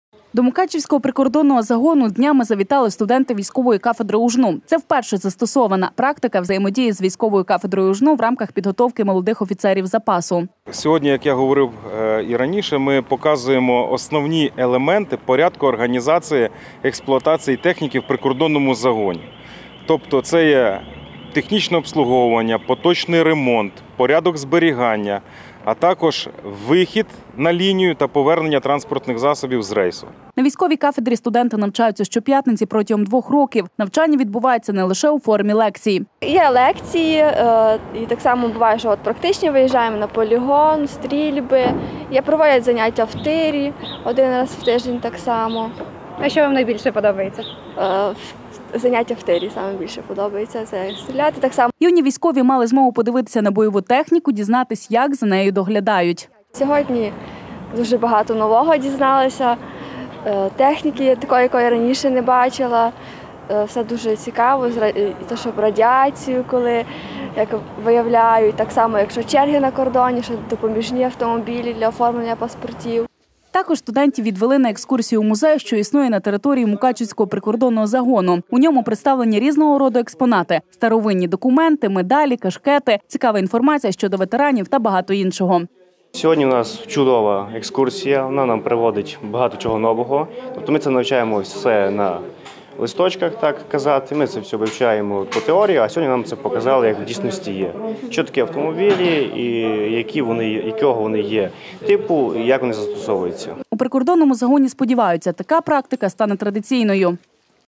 Бойову техніка прикордонників продемонстрували студентам
Студенти військової кафедри УжНУ днями завітали до Мукачівського прикордонного загону. Тут для них провели екскурсію, продемонстрували бойову техніку, та показали як за нею доглядають. До Мукачівського прикордонного загону днями завітали студенти військової кафедри УжНУ. Це вперше застосована практика взаємодії з військовою кафедрою УжНУ в рамках підготовки молодих офіцерів запасу.На військовій кафедрі студенти навчаються щоп’ятниці протягом двох років. Навчання відбувається не лише у формі лекцій. Студенти мали змогу подивитися на бойову техніку, дізнатись як за нею доглядають. Також студентів відвели на екскурсію в музей, що існує на території Мукачівського прикордонного загону.